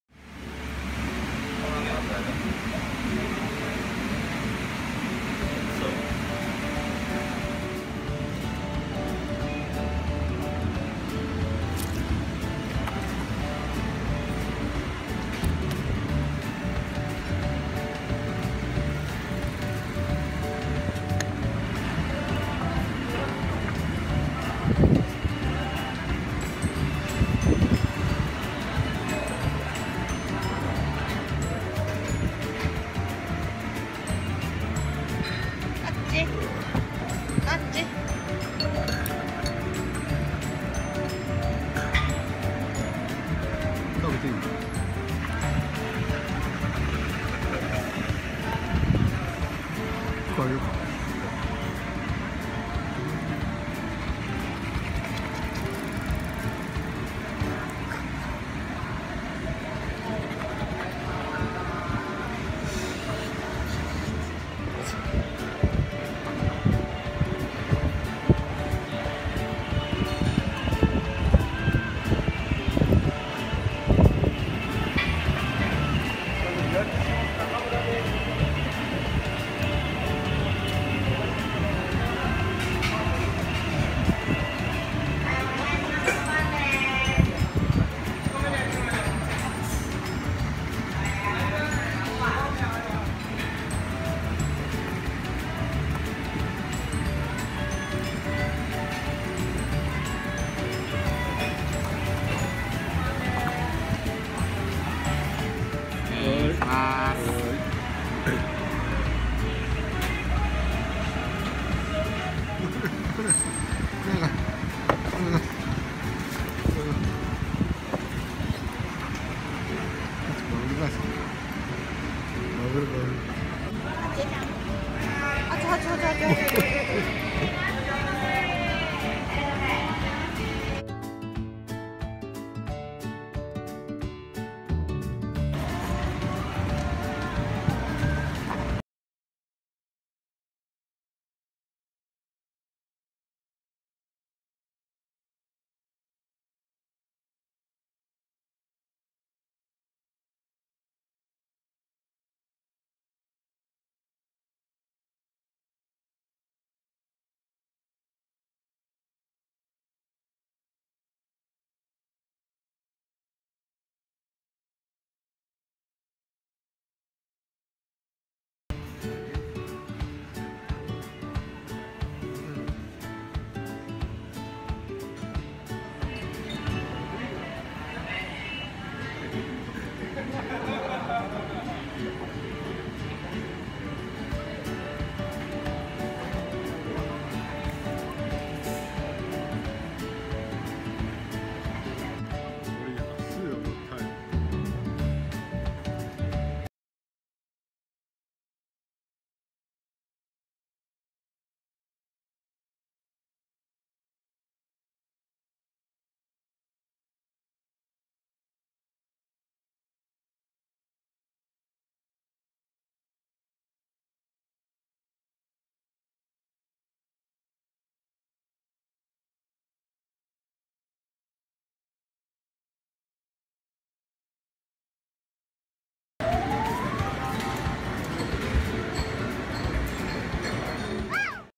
イソノサザエさんでお魚を満喫したゴールデンレトリバーのはっち
昨年の夏、屋外にあるテーブル席を見て、店員さんにワンコ連れでいいか確認した所、快く席に案内してくださいました。お通しは、はっちの為に味付けなしのキャベツをすぐに出してくださいます。とても有り難いことなので、迷惑をかけないように、開店時間に行くようにしています。決しておごることなく、はっちと居酒屋を楽しめたらいいなぁと願っています。
ゴールデンレトリバー女の子6歳